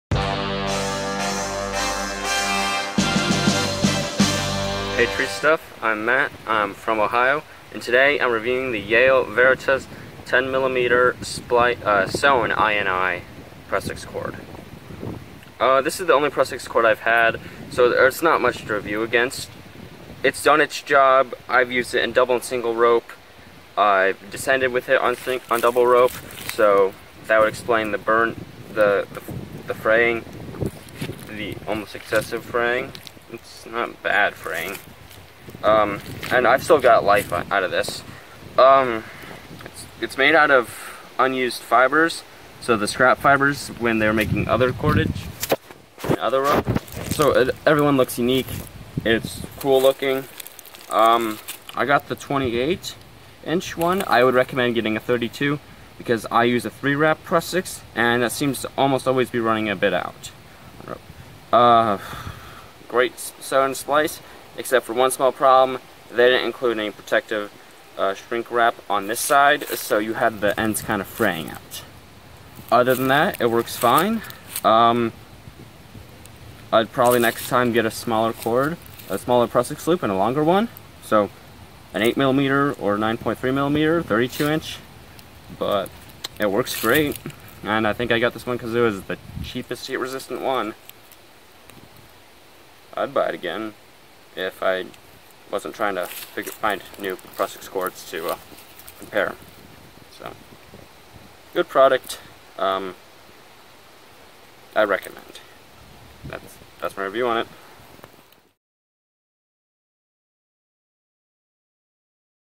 Yale Veritas 10mm Sewn Eye and Eye - TreeStuff.com Customer Matt Humphries' Review In The Field
Ohio tree climber Matt Humphries gives us his Review In The Field of the Yale Veritas 10mm Sewn Eye and Eye available

For info on how to make money reviewing your favorite TreeStuff products head